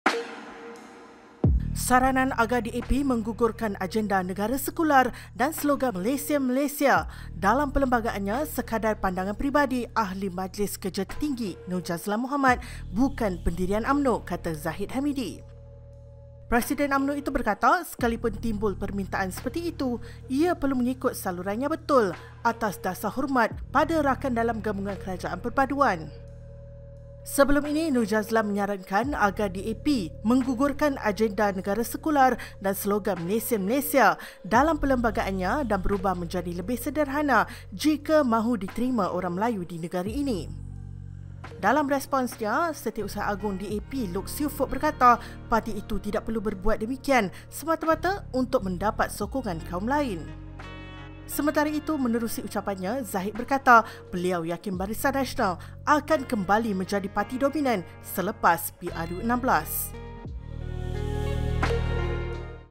Gesa DAP gugur slogan ‘Malaysian Malaysia’ hanya pandangan peribadi Nur Jazlan, kata Zahid
Saranan agar DAP menggugurkan agenda negara sekular dan slogan “Malaysian Malaysia” dalam perlembagaannya sekadar pandangan peribadi ahli Majlis Kerja Tertinggi (MKT) Nur Jazlan Mohamed, bukan pendirian Umno, kata Zahid Hamidi.